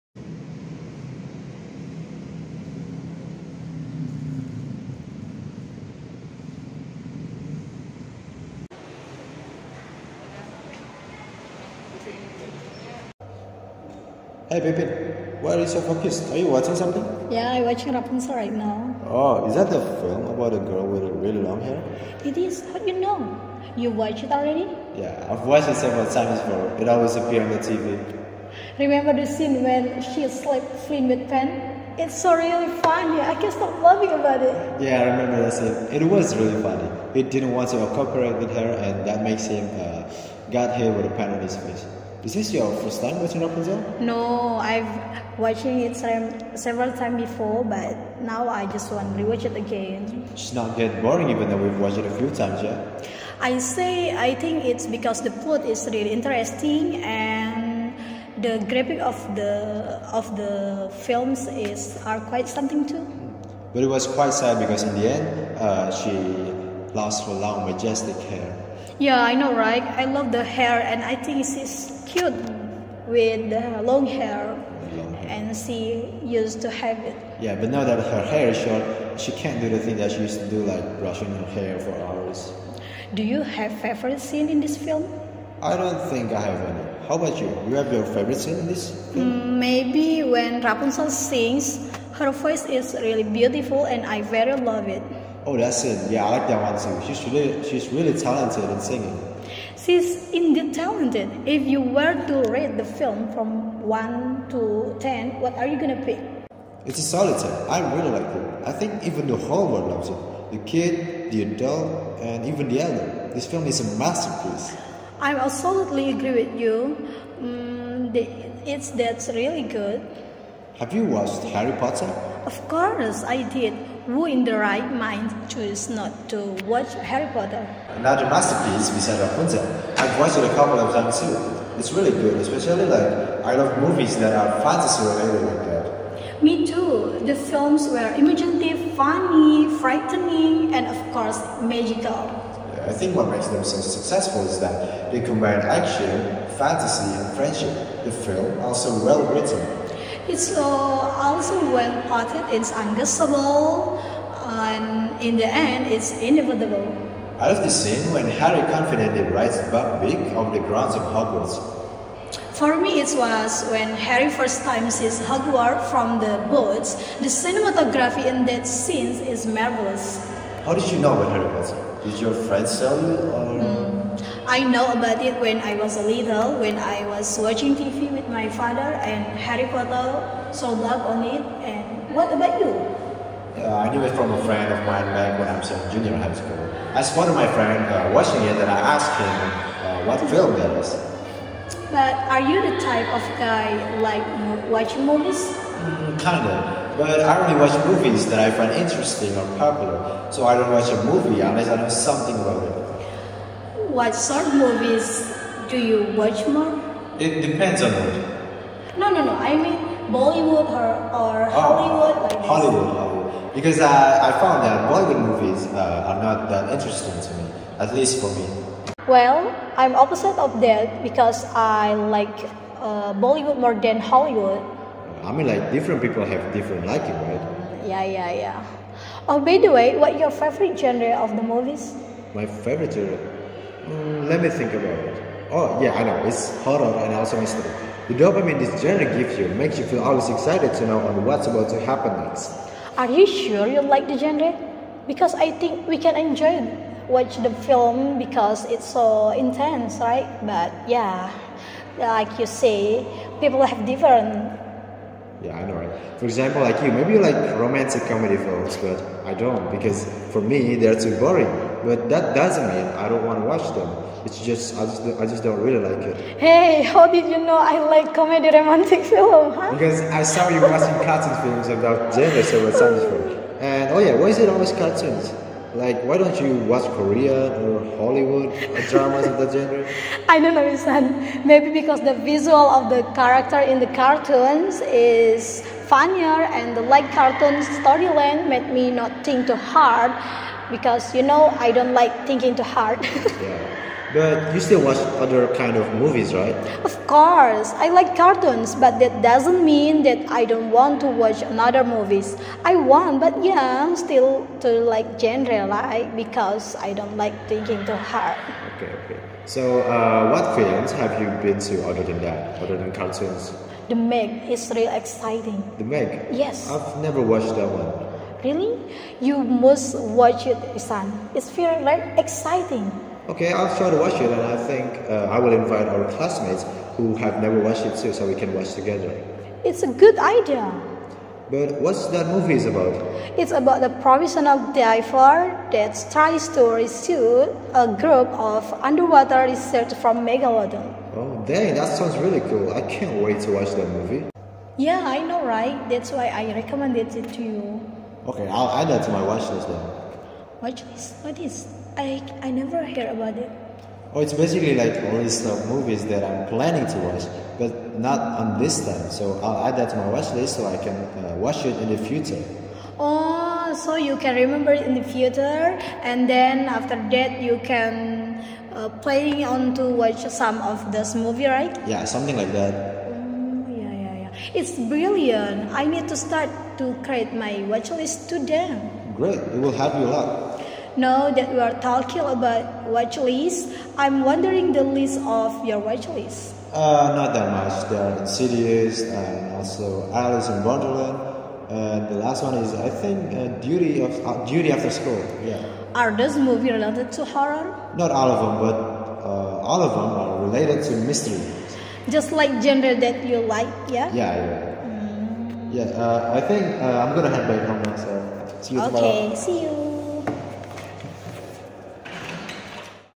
Muhammad Ihsan & Nur Reski Pepin | MidTerm Exam - Interactional Conversation
video ini dibuat guna menyelesaikan tugas ujian tengah semester.

present to you by:
1. Muhammad Ihsan Arifin Zuhdi
2. Nur Rezki Amelia Pepin Hasibuan